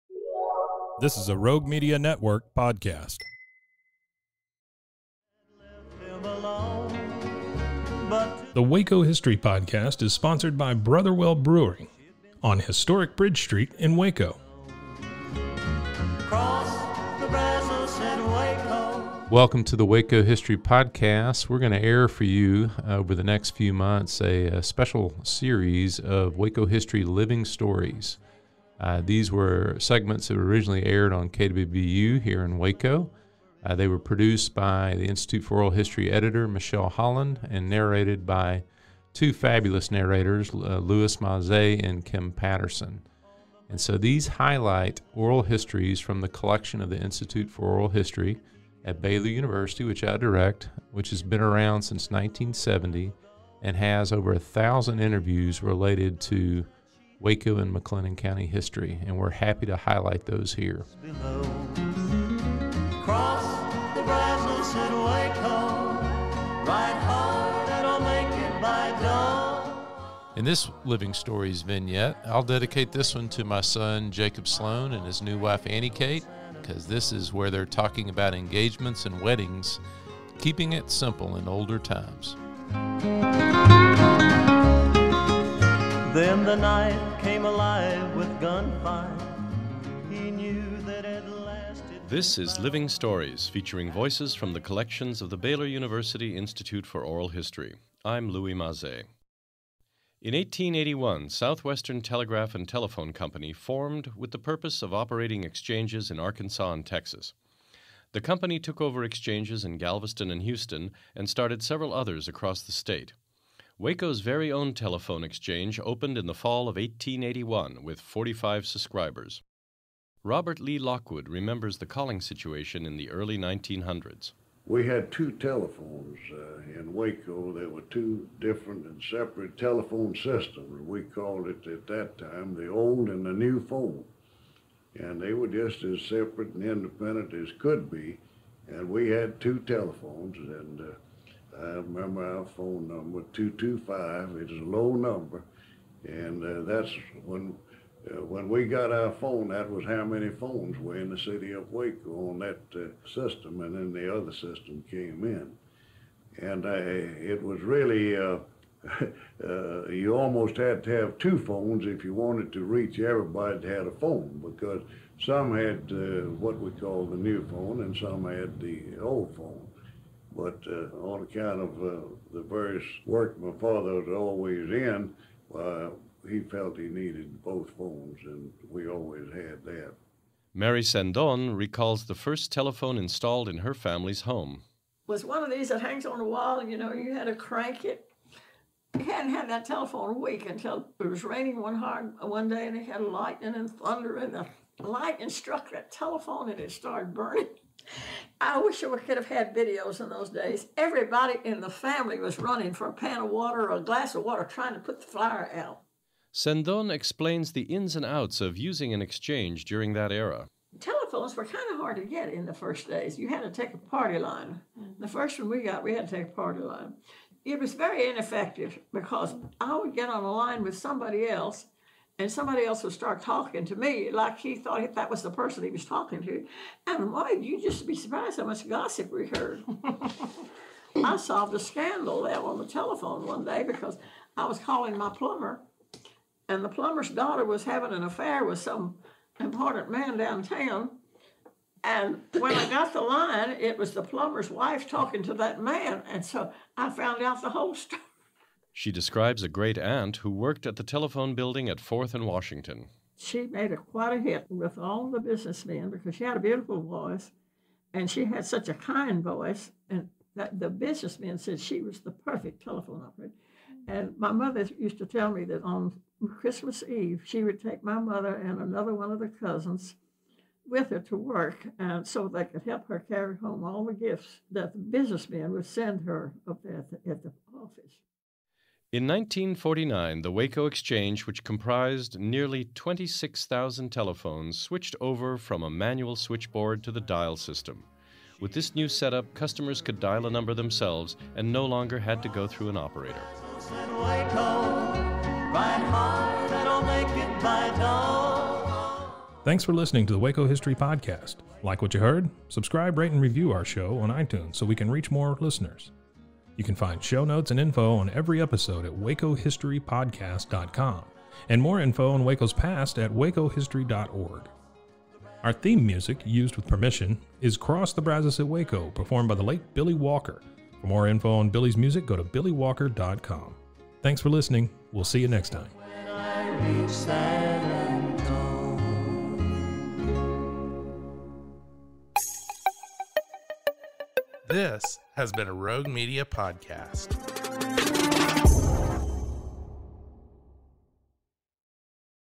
Living Stories: Early Telephone Service in Waco | Waco History Podcast
This is Living Stories, featuring voices from the collections of the Baylor University Institute for Oral History. I'm Louis Mazé.
In 1881, Southwestern Telegraph and Telephone Co. formed with the purpose of operating exchanges in Arkansas and Texas. The company took over exchanges in Galveston and Houston and started several others across the state. Waco's very own telephone exchange opened in the fall of 1881 with 45 subscribers.
Robert Lee Lockwood remembers the calling situation in the early 1900s:
"We had two telephones in Waco. There were two different and separate telephone systems. We called it at that time ‘the old and the new phone.' And they were just as separate and independent as could be. It was a low number. And that's when—when we got our phone, that was how many phones were in the city of Waco on that system, and then the other system came in. And it was really—you almost had to have two phones if you wanted to reach everybody that had a phone because some had what we called ‘the new phone' and some had ‘the old phone.' But on account of the various work my father was always in, why, he felt he needed both phones, and we always had that."
Mary Sendón recalls the first telephone installed in her family's home:
"Was one of these that hangs on the wall; you know, you had to crank it. We hadn't had that telephone a week until it was raining hard one day, and they had lightning and thunder. And lightning struck that telephone, and it started burning. (laughs) I wish we could have had videos in those days. Everybody in the family was running for a pan of water or a glass of water trying to put the fire out."
Sendón explains the ins and outs of using an exchange during that era:
"Telephones were kind of hard to get in the first days. You had to take a party line. The first one we got we had to take a party line. It was very ineffective because I would get on a line with somebody else, and somebody else would start talking to me like he thought that was the person he was talking to. And, boy, you'd just be surprised how much gossip we heard! (interviewer laughs) I solved a scandal there on the telephone one day because I was calling my plumber, and the plumber's daughter was having an affair with some important man downtown. And when I got the line, it was the plumber's wife talking to that man, and so I found out the whole story. (laughs)"
She describes a great-aunt who worked at the telephone building at Fourth & Washington:
"She made a—quite a hit with all the businessmen because she had a beautiful voice and she had such a kind voice that the businessmen said she was the perfect telephone operator. And my mother used to tell me that on Christmas Eve she would take my mother and another one of the cousins with her to work so they could help her carry home all the gifts that the businessmen would send her up there at the office."
In 1949, the Waco exchange, which comprised nearly 26,000 telephones, switched over from a manual switchboard to the dial system. With this new setup, customers could dial a number themselves and no longer had to go through an operator.
With early crank telephones like this one, users turned the handle on the side to ring for an operator.